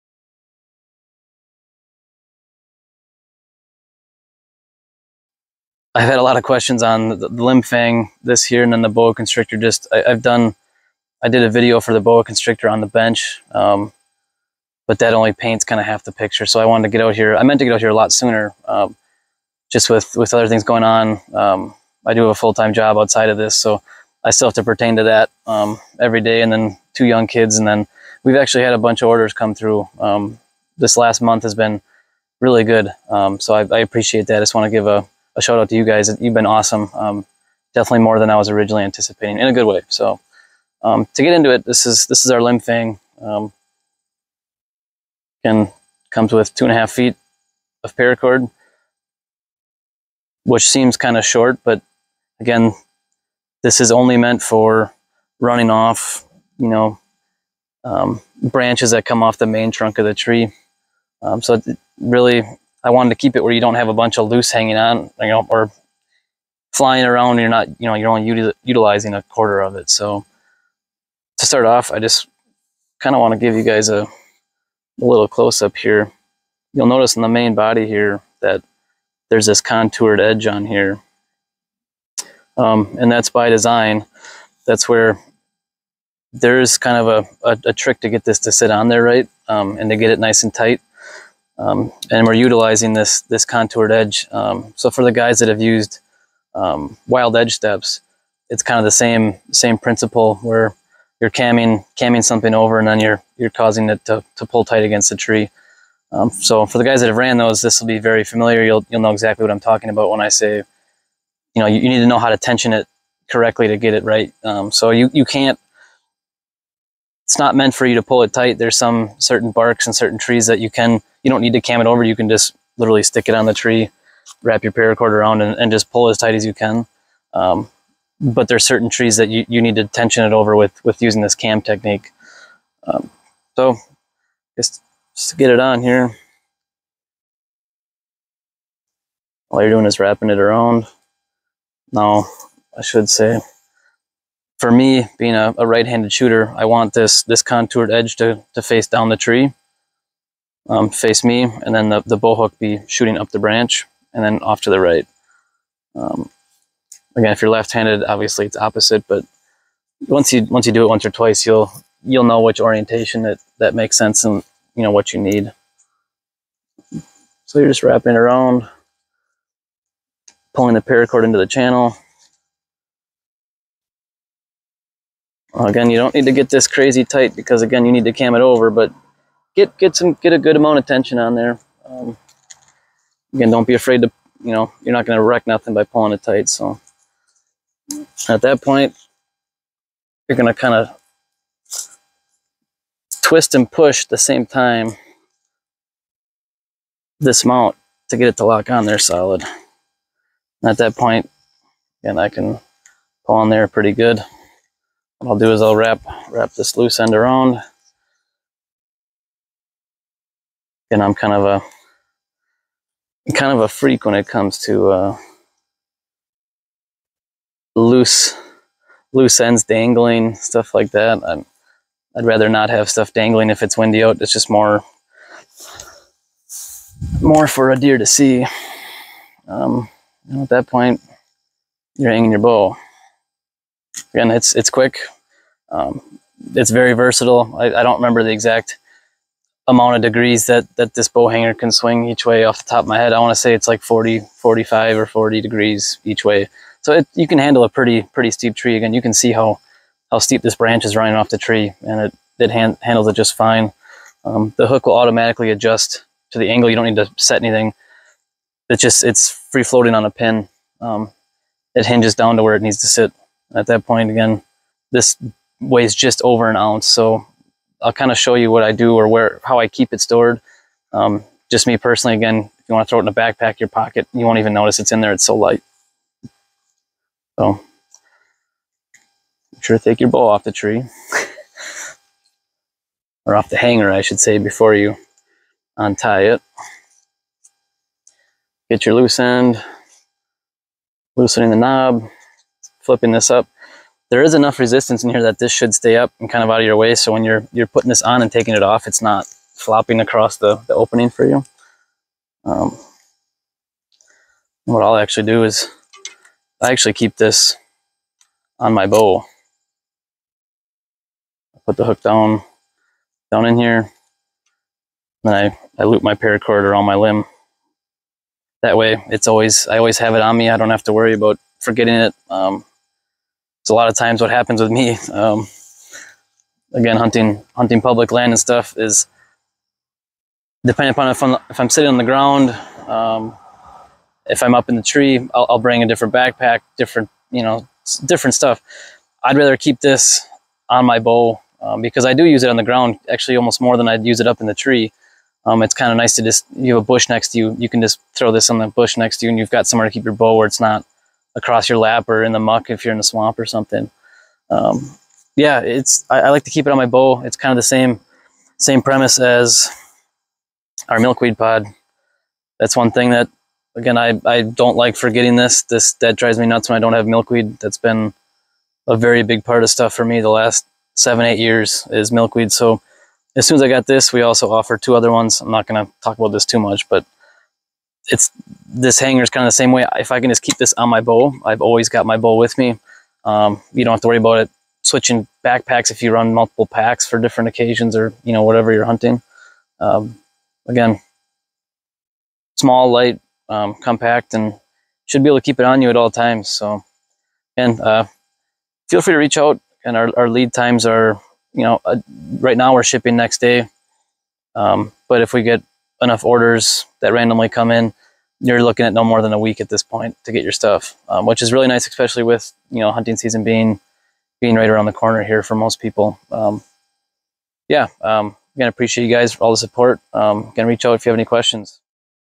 Limb Fang Bow Hanger Overview
In this video I go over our Limb Fang bow hanger, designed to work with limbs or branches that come off the main trunk of the tree. This mount weighs just over 1oz and is completely public land legal.